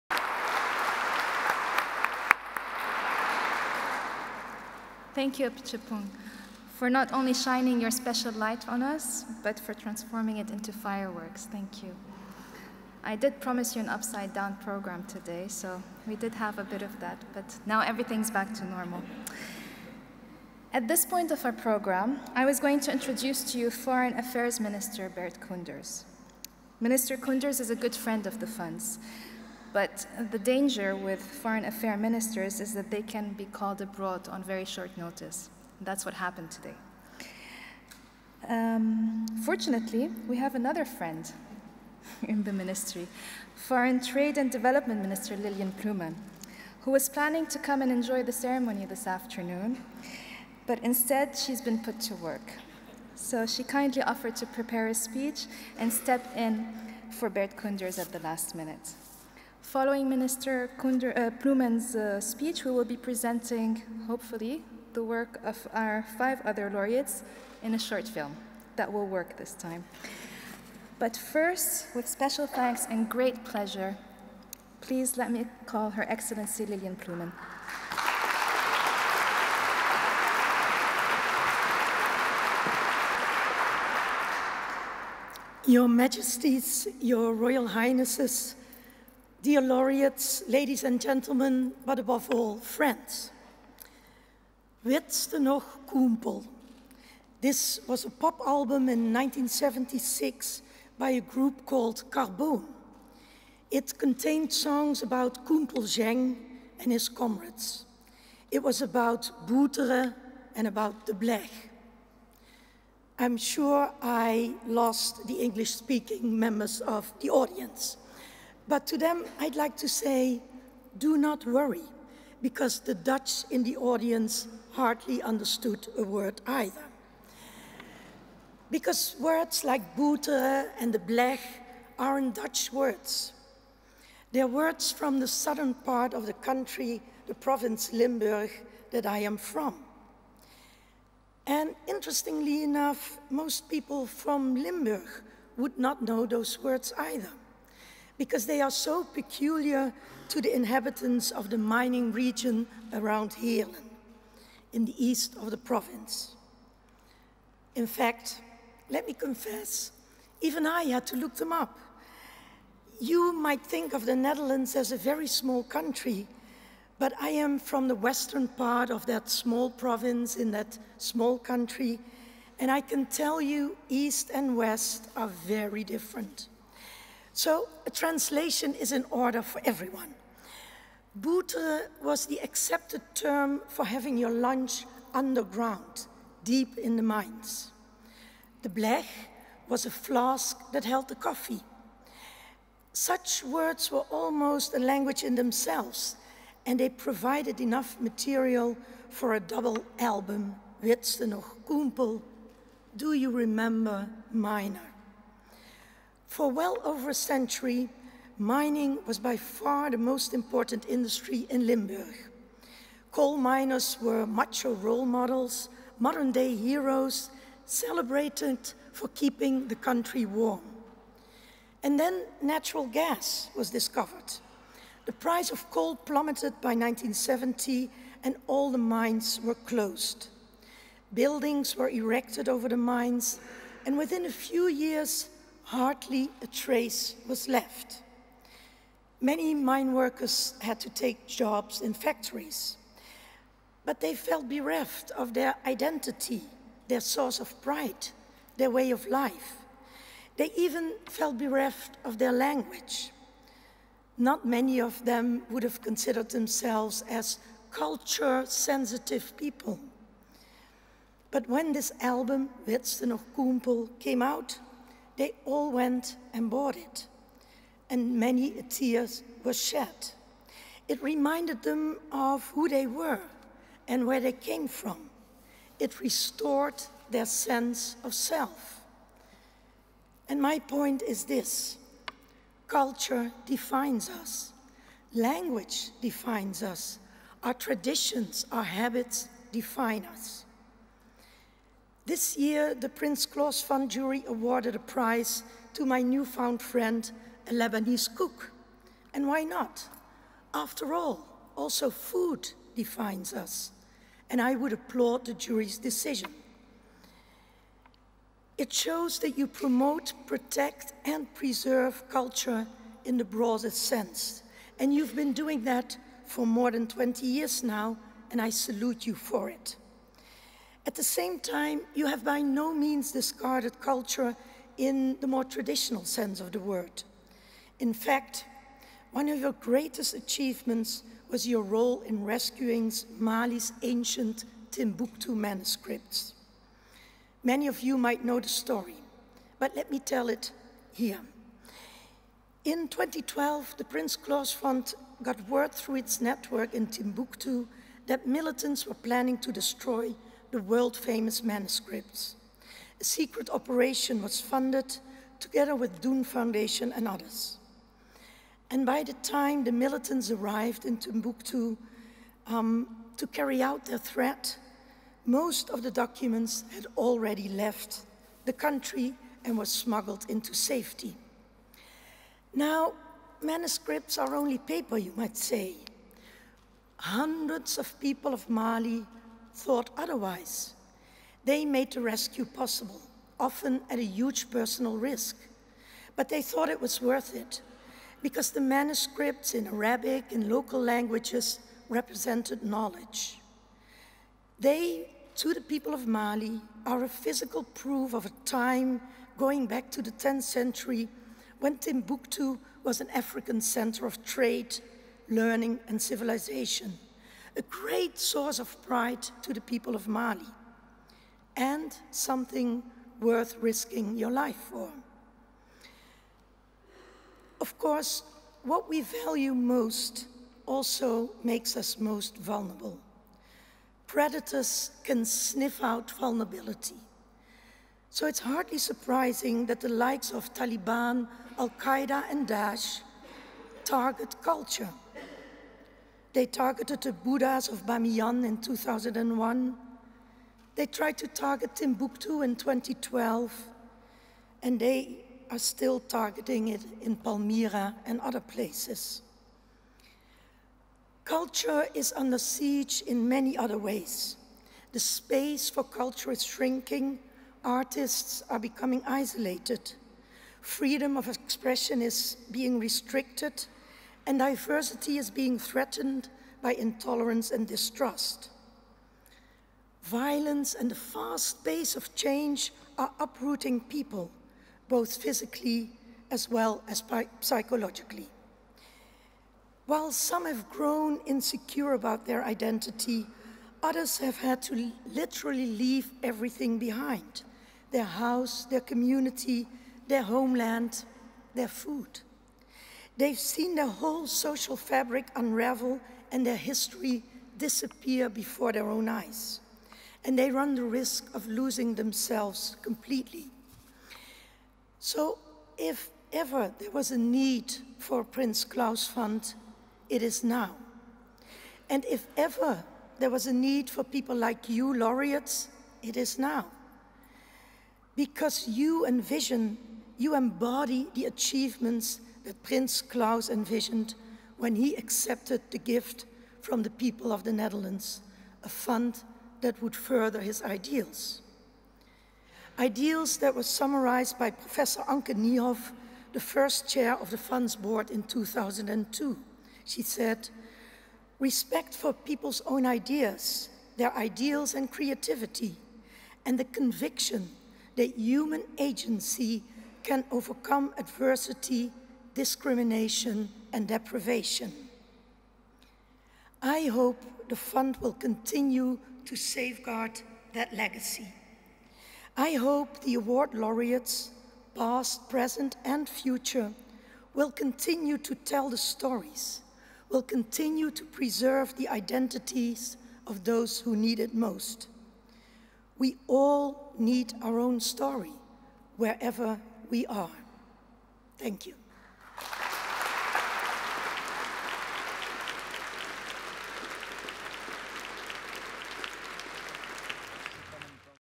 Speech by Minister Lilianne Ploumen
On 15 December at a ceremony at the Royal Palace Amsterdam, HRH Prince Constantijn of the Netherlands presented six Prince Claus Awards for outstanding achievement in the field of culture and development.

The Principal Prince Claus Award went to independent filmmaker Apichatpong Weerasethakul (Thailand). The five additional Prince Claus Awards were presented to chef and food activist Kamal Mouzawak (Lebanon), interdisciplinary cultural centre PeaceNiche | The Second Floor (T2F) (Pakistan), graphic designer, artist, educator and historian Bahia Shehab (Egypt/Lebanon), the digital portal La Silla Vacía (Colombia) and architect Vo Trong Nghia (Vietnam).

The Awards Ceremony included a screening of the short film installation, Fireworks by Principal Prince Claus Laureate Apichatpong Weerasethakul, performances by the Syrian musician Kinan Azmeh and artist Kevork Mourad, and speeches by Apichatpong Weerasethakul, Honorary Chair of the Board HRH Prince Constantijn, and Minister Lilianne Ploumen.